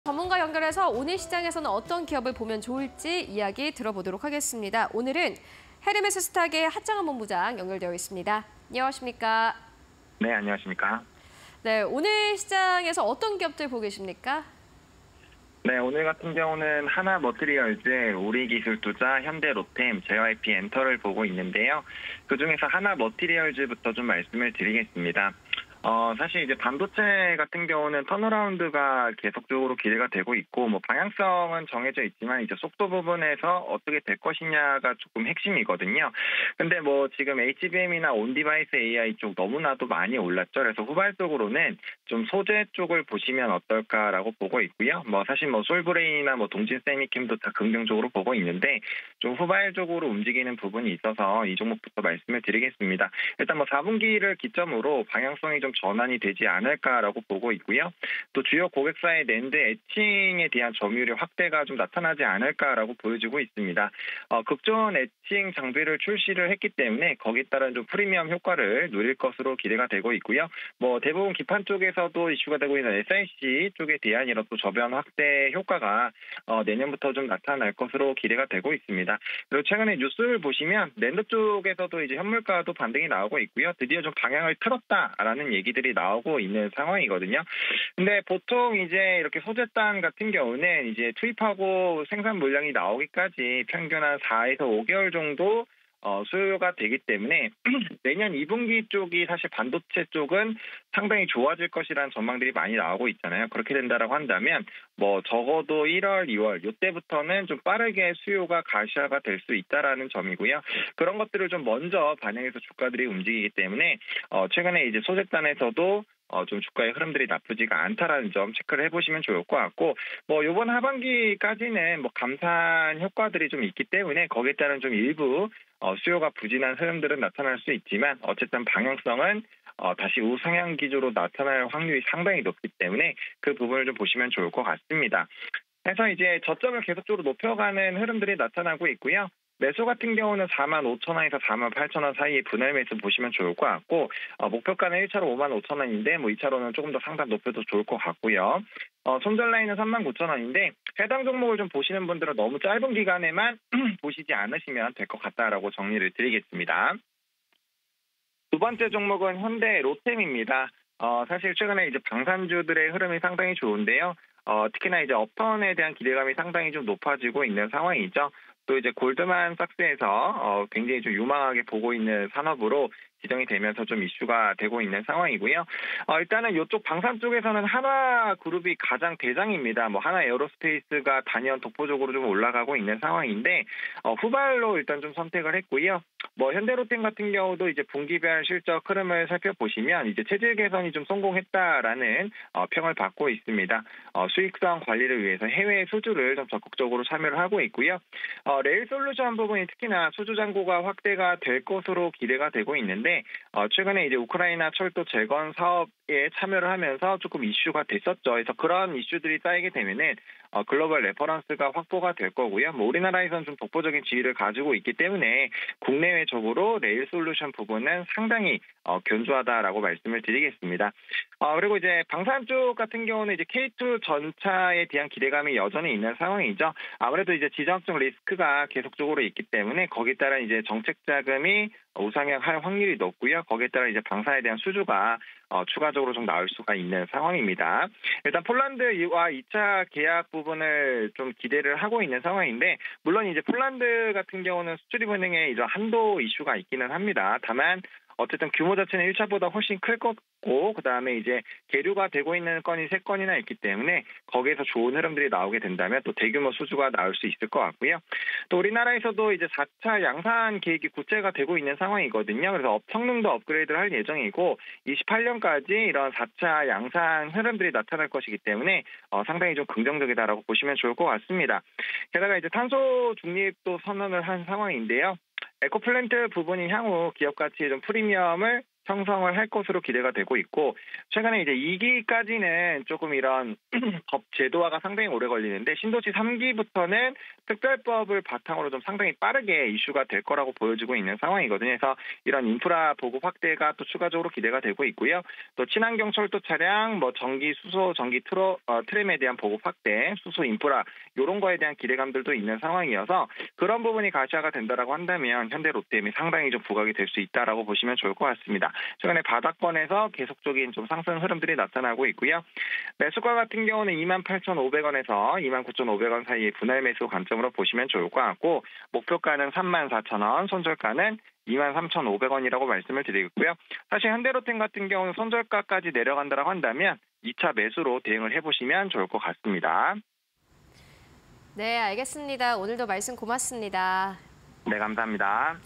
하나머티리얼즈, 우리기술투자, 현대로템, JYP Ent._하창완의 주력 종목 (20231207)
# 하창완의 주력 종목
1. 하나머티리얼즈(166090)
2. 우리기술투자(041190)
3. 현대로템(064350)
4. JYP Ent. (035900)

하나머티리얼즈(166090)
- 실리콘(Sic) 및 실리콘 소모성 부품 생산
- 실리콘 카바이드링 등 제품 주력
- 실리콘 부품 수요 증가 예상
- 주요 고객사의 낸드 점유율 확대
- 극저온 식각 장비 출시 효과 기대
- D램 및 낸드, 내년 턴어라운드 기대

- 수익성 관리 위해 해외 수주 적극 참여
- 레일솔루션 부문서 수주 잔고 확대
- 분기별 실적 흐름 따라 체질 개선 성공
- K-2 전차 수요 증가세
- 폴란드와 2차 계약 최종 협상 진행
- 국내 K-2 실전 배치 위한 4차 양산 계획
- 2028년까지 약 150대 양산 기대